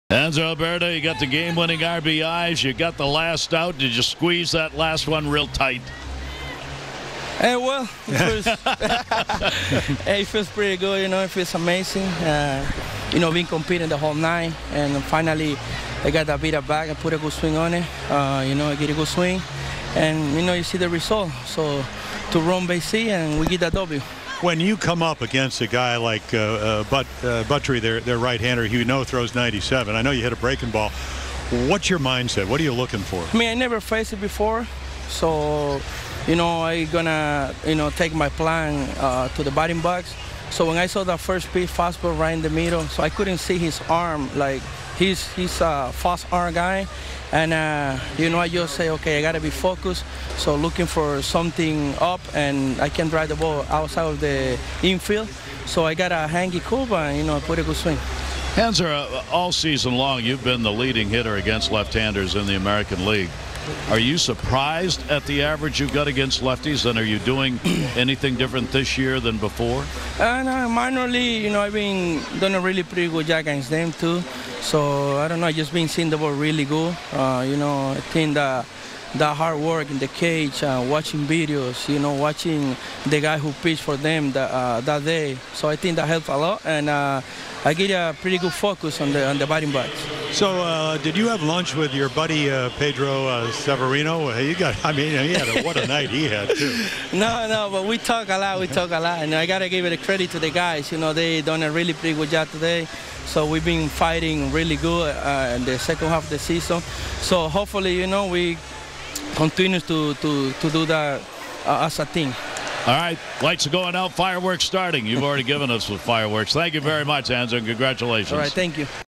Hanser Alberto after O's 8-7 win in Anaheim
Hanser Alberto stopped by the MASN broadcast booth after the Orioles defeated the Angels on Saturday night. More